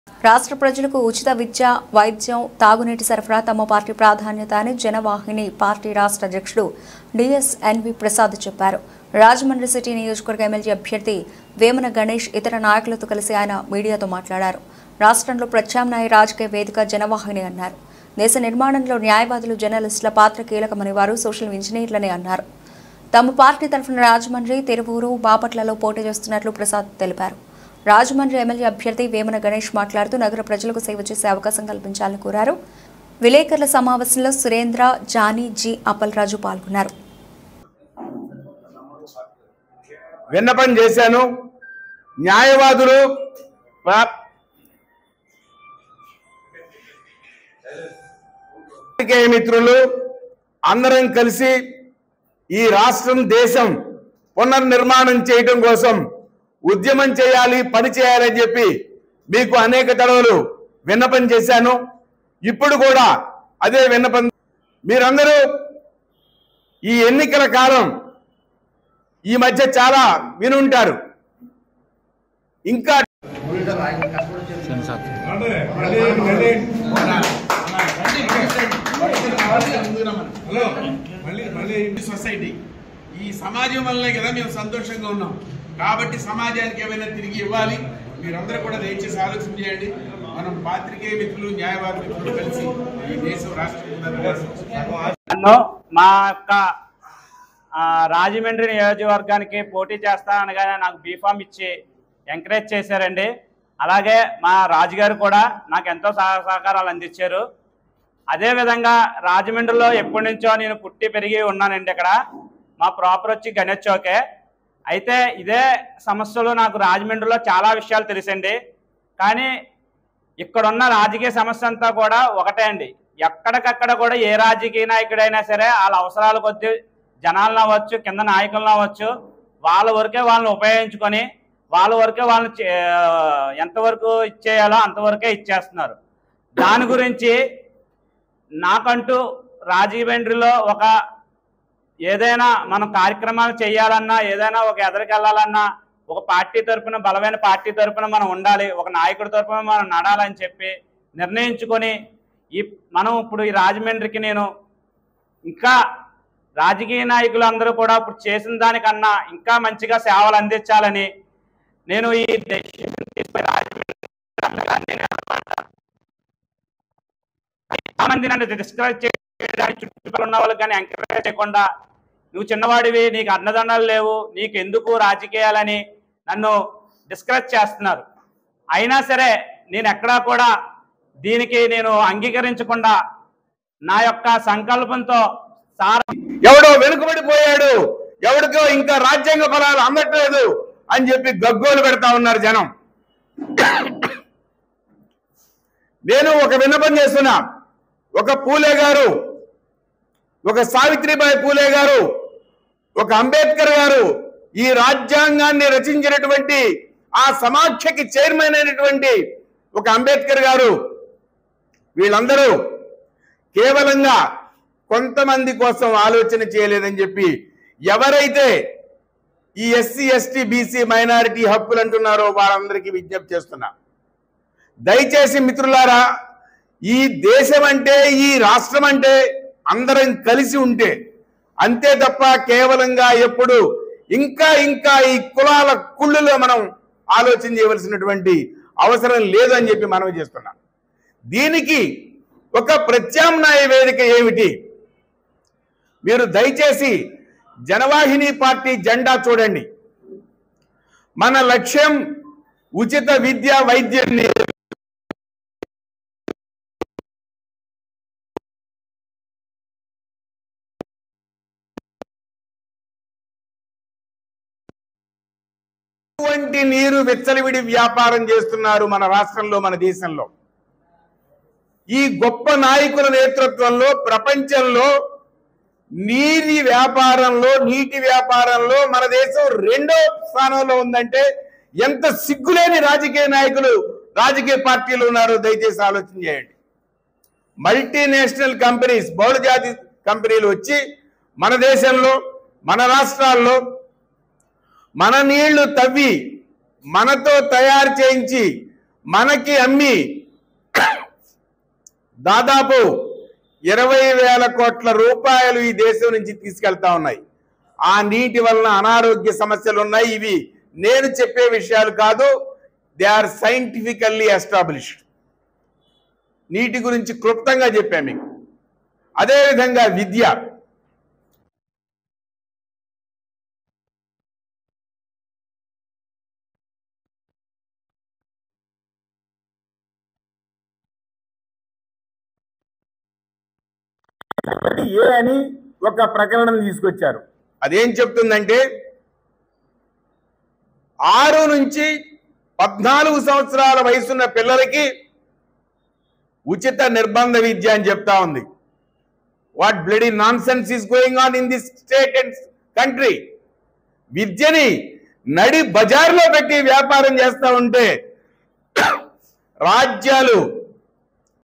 ABC NEWS TELUGU//ఉచిత విద్య, వైద్యం, తాగునీటి సరఫరా తమపార్టీలక్ష్యం:జనవాహిని అధ్యక్షుడు.
ఉచిత విద్య, వైద్యం, తాగునీటి సరఫరా తమ పార్టీ లక్ష్యం

జనవాహిని అధ్యక్షుడు రాష్ట్ర ప్రసాద్

రాజమహేంద్రవరం:
రాష్ట్ర ప్రజలకు ఉచిత విద్య వైద్యం తాగునీటి సరఫరా తమ పార్టీ ప్రాధాన్యత అని జనవాహిని పార్టీ రాష్ట్ర అధ్యక్షుడు డి.ఎస్.ఎన్.వి.ప్రసాద్ చెప్పారు.రాజమండ్రి సిటీ నియోజకవర్గ ఎమ్మెల్యే అభ్యర్థి వేమన గణేష్,ఇతర నాయకులతో కలిసి ఆయన మీడియాతో మాట్లాడారు.రాష్టంలో ప్రత్యామ్నాయ రాజకీయ వేదిక జనవాహిని అన్నారు.దేశ నిర్మాణంలో న్యాయవాదులు, జర్నలిస్టుల పాత్ర కీలకమని వారు సోషల్ ఇంజనీర్లని అన్నారు.తమ పార్టీ తరపున రాజమండ్రి, తిరువూరు, బాపట్లలో పోటీ చేస్తున్నట్లు ప్రసాద్ చెప్పారు.రాజమండ్రి ఎమ్మెల్యే అభ్యర్థి వేమన గణేష్ మాట్లాడుతూ నగర ప్రజలకు సేవ చేసే అవకాశం కల్పించాలని కోరారు.విలేకరుల సమావేశంలో సురేంద్ర,జాని,జి.అప్పలరాజు పాల్గొన్నారు.